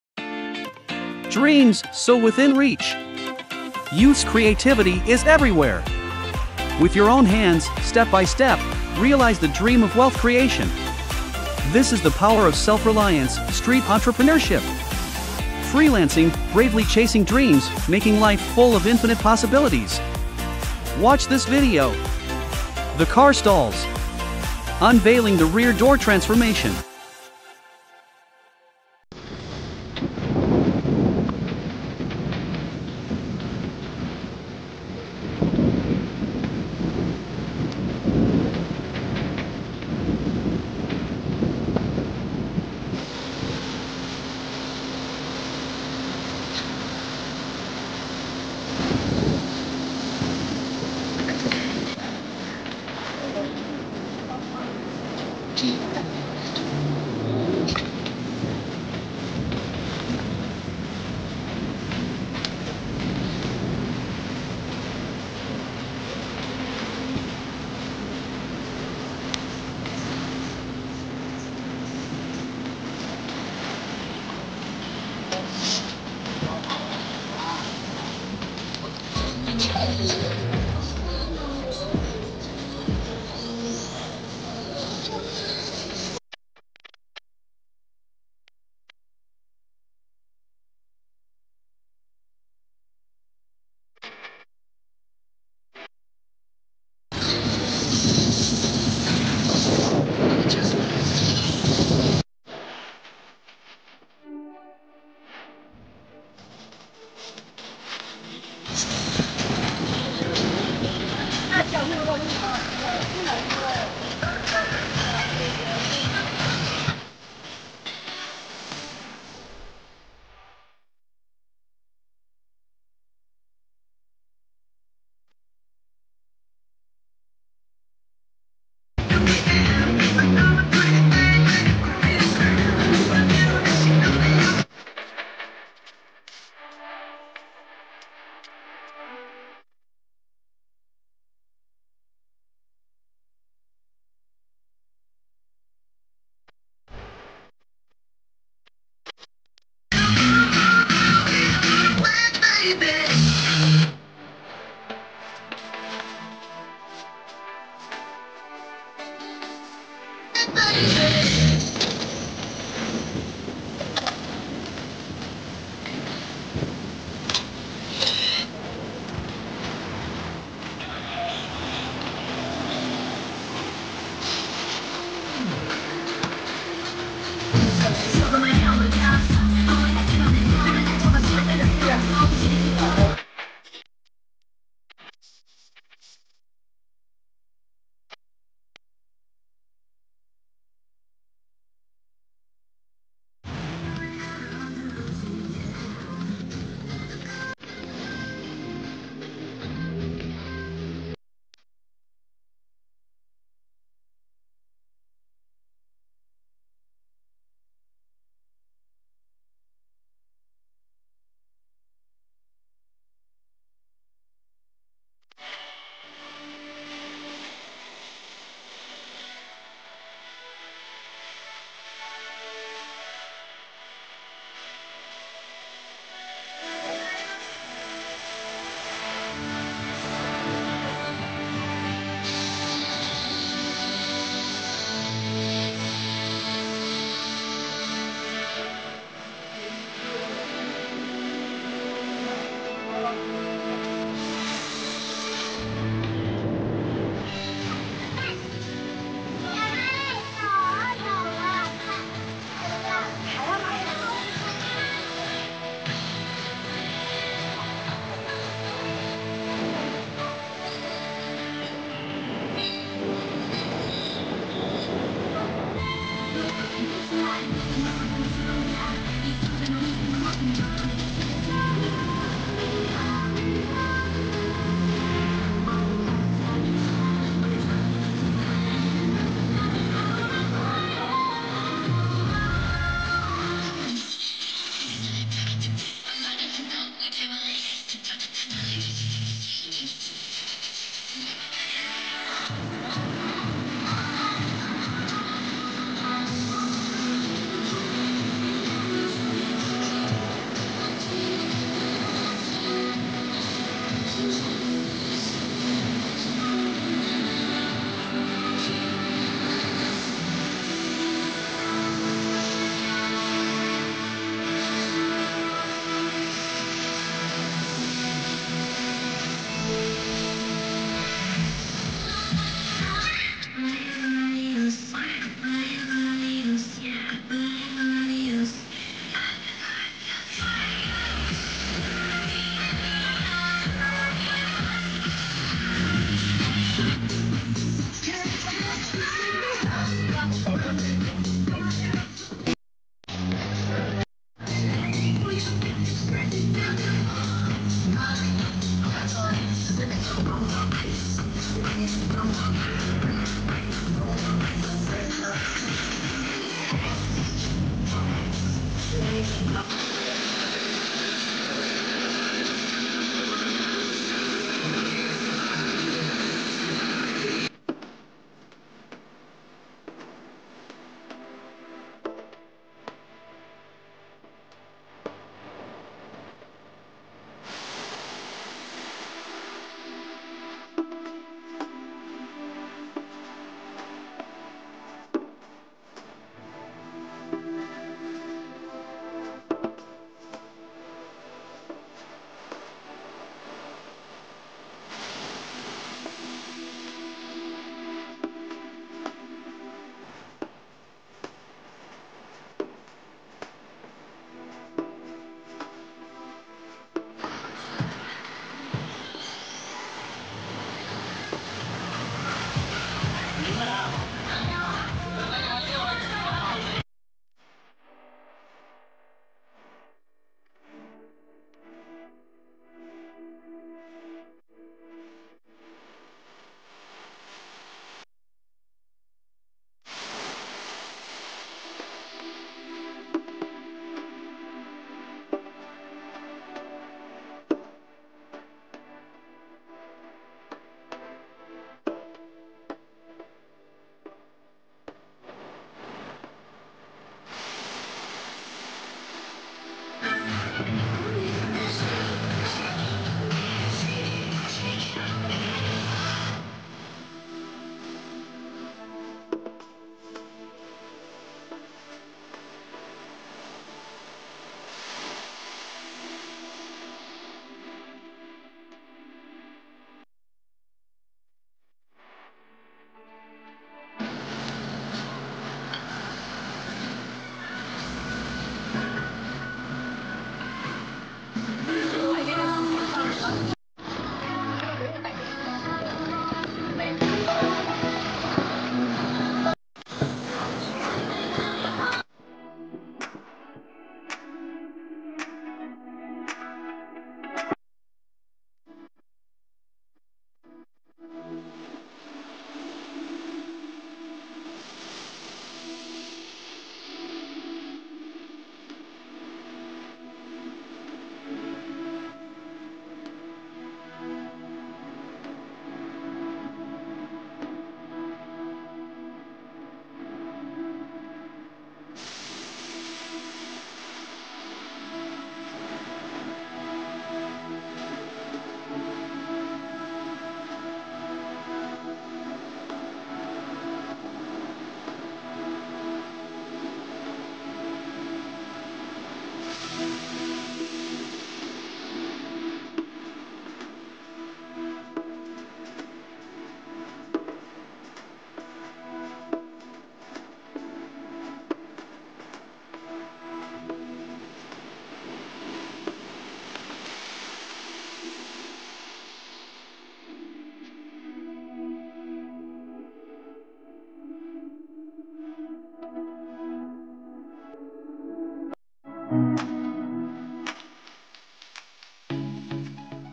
Car Stall Street Business: Empowering Freelancers with Rear Door Entrepreneurship!
00:00 intro
00:31 Stall setting up, preparation work
05:36 Night time party, making money

How to make money? In this video, we introduce a revolutionary business concept - Car Tailgate Business! This innovative approach empowers freelancers to harness their entrepreneurial spirit and achieve self-sufficiency. Learn how to creatively transform your car's rear door into a dynamic marketplace, offering a diverse range of stall businesses. From pop-up shops to mobile cafes, this video provides valuable insights and inspiration for utilizing your car's rear door as a thriving entrepreneurial hub.

Some related stuffs in this video can be recommended